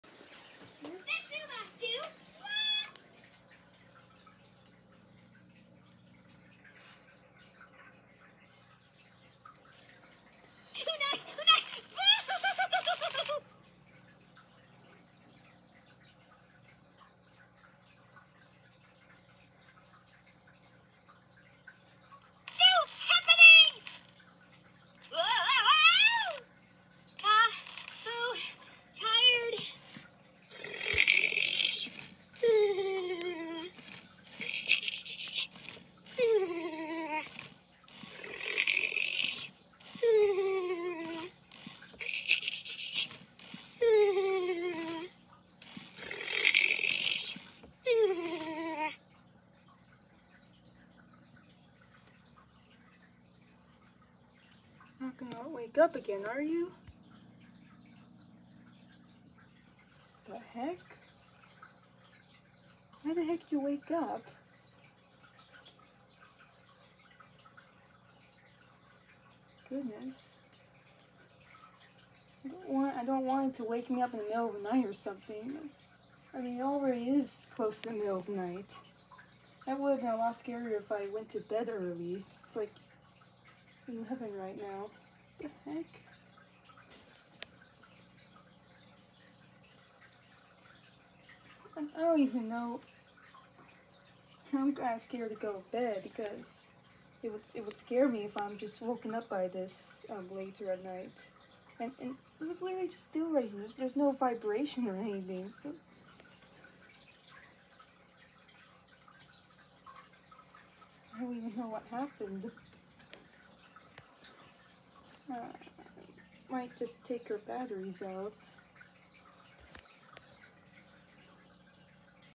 My Furby Boom woke up by herself and I don’t know why
I’ve had this Furby for many years, since I was little, and this is the first time I had this happen without explanation and my reaction to it. Her tilt sensor is really sensitive, but she wasn’t moving in the slightest. No vibration or anything is over there. It’s perfectly still. Luckily, though it was 11 PM (it’s still late as I’m uploading this), I wasn’t trying to sleep. I was awake watching a video with the light on, so when I started hearing snoring I was so confused and wondering if it was coming from the video. I was shocked the Boom was awake, and was worried it would wake up my parents. Her yelling woke up my Aibo ERS-1000 Roxy and my cats.

I get startled by unusual happenings and sounds at night thanks to wacky nightmares I suffered when little, so was spooked and jumpy. It would’ve been worse if I’d already had the light off and was trying to sleep, good thing I wasn’t. I’m concerned she’ll do it again now and want to remove the batteries.

If someone knows why this happened, I’d like an explanation. I thought 1998s were only really known for pulling this off, and my 1998 has his batteries out, but I guess now I got to be wary of Booms too. Whatever caused the issue here, it would’ve been nicer if it had been in the day or at least earlier evening so I would’ve been better prepared.

If the comments get disabled because YouTube doesn’t like a 20 year old female (that’s me) around Furbies for some reason (especially when I talk), just comment on the community post I made for this nonsense. Or if something somehow happens to the community post, comment on a different video.